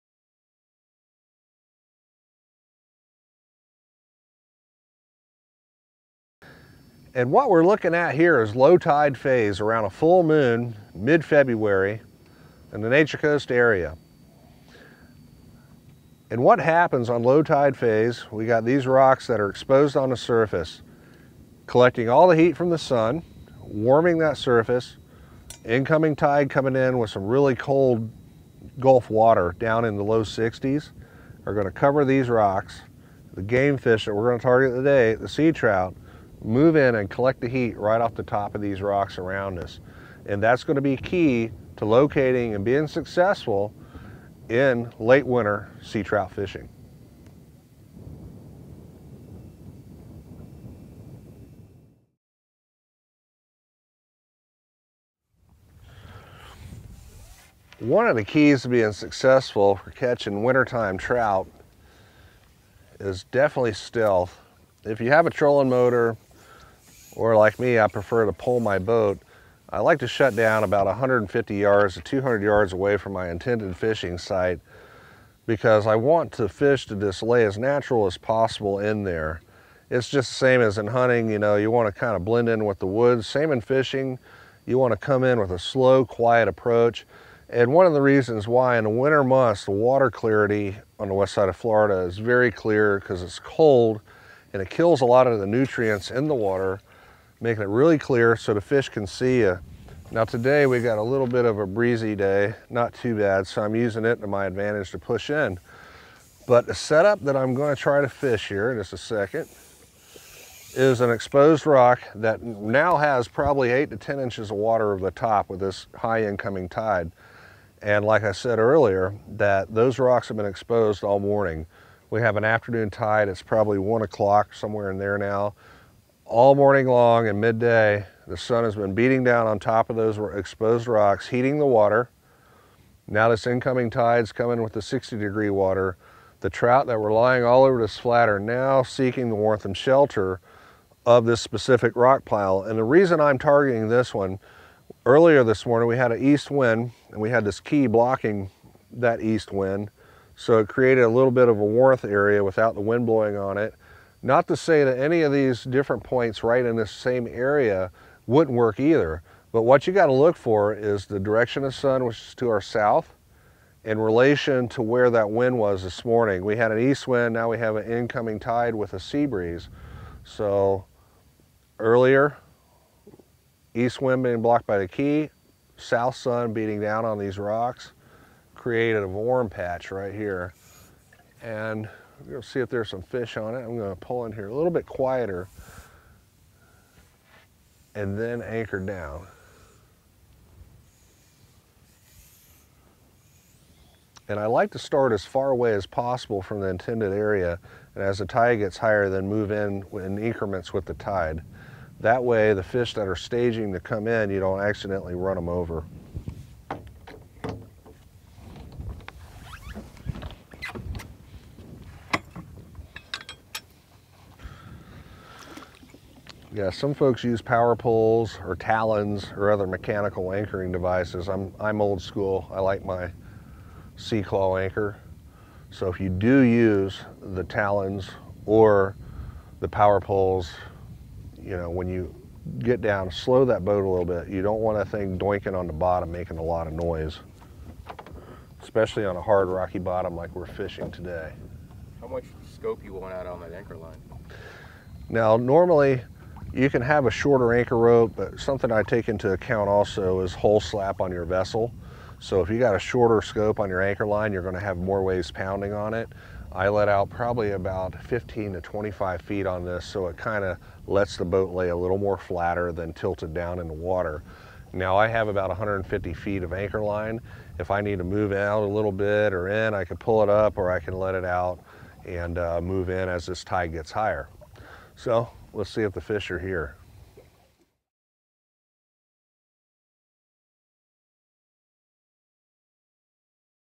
Bangin' Out Winter Sea Trout | Florida Fishing - In The Spread Fishing Instruction
Fishing Knowledge = Success

Spotted sea trout or speckled trout are a species that you can target year round in Florida. One of the more challenging times of year will be in the winter when cold Gulf of Mexico waters can push the fish off the keys and limestone rocks out to deeper water. The key is to fish areas that are exposed to the sun on outgoing or low tides. These areas absorb warmth from the sun. When the tides turns and pushes water in covering these areas, speckled trout will school up to take advantage of the warmth and feed on food being pushed with the incoming water. Understanding this goes a long way to narrowing down where to start fishing.

You may purchase any of our full length videos on DVD from the In The Spread store on Amazon

In The Spread films and videos are produced by Seth Horne in association with Full Throttle Media.